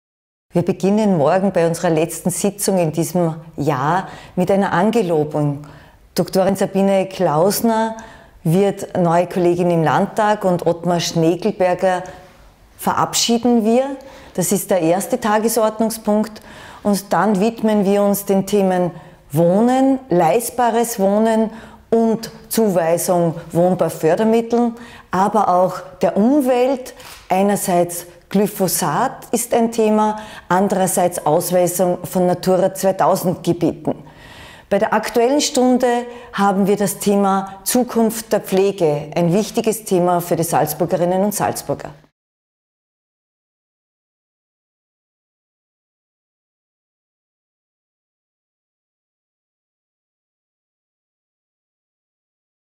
Ankündigung Landtagsplenum 20.12.
"Die Zukunft der Pflege – Chancen und Herausforderungen" lautet das Thema der aktuellen Stunde bei der Plenarsitzung des Salzburger Landtags am 20. Dezember 2017. Weiters wird eine neue Landtagsabgeordnete angelobt und es werden unter anderem Anfragen zu den Themen Wohnen und Natur behandelt.